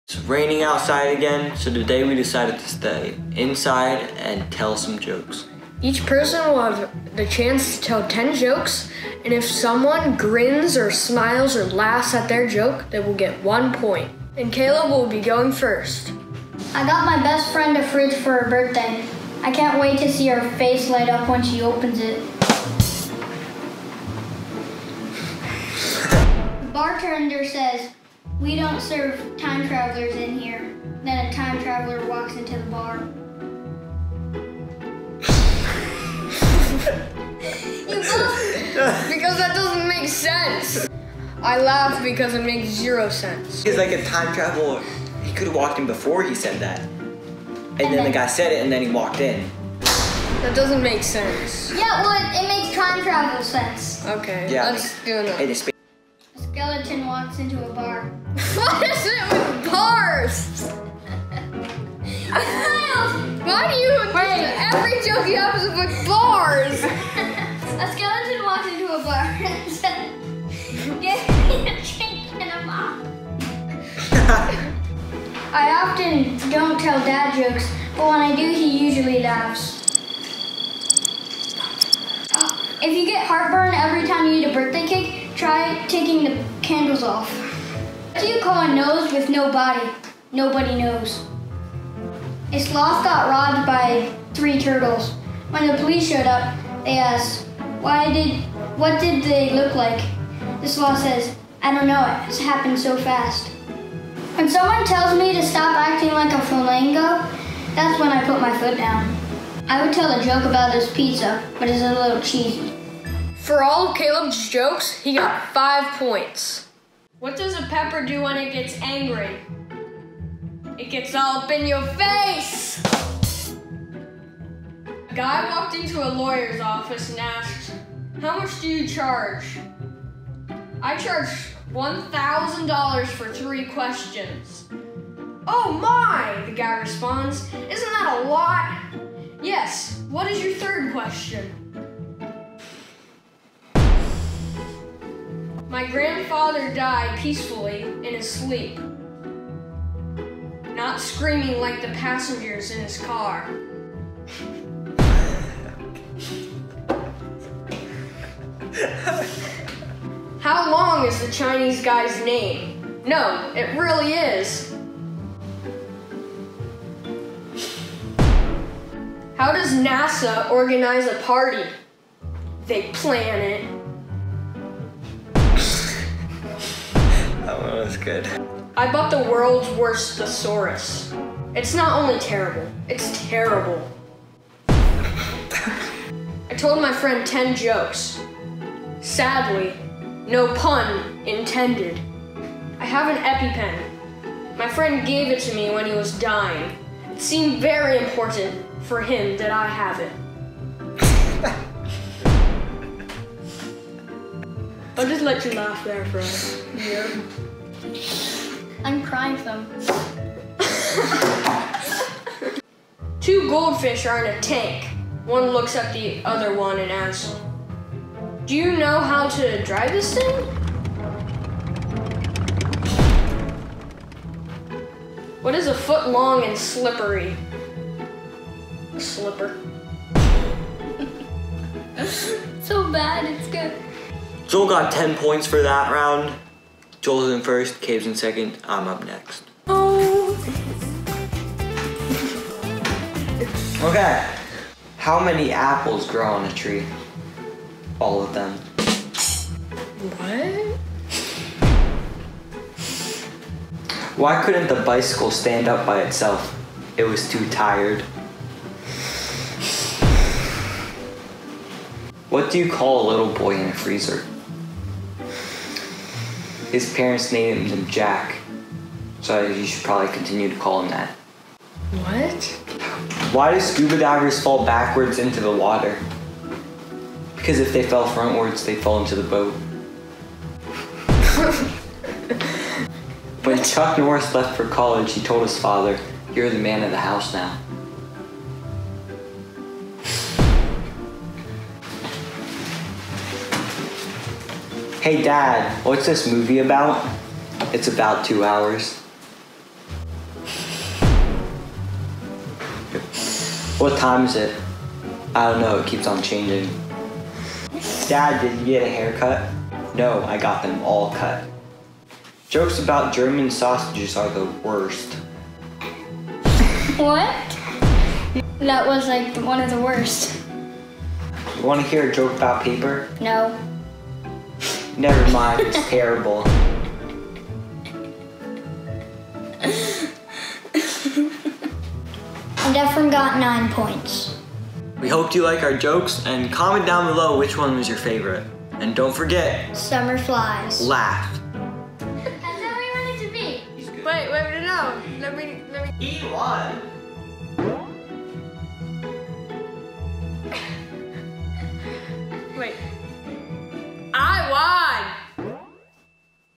Try Not To Laugh
Another rainy day! Each boy gets 10 jokes to try and make his brothers laugh. They get 1 point each time someone laughs. Watch and see who wins and who tells the worst jokes.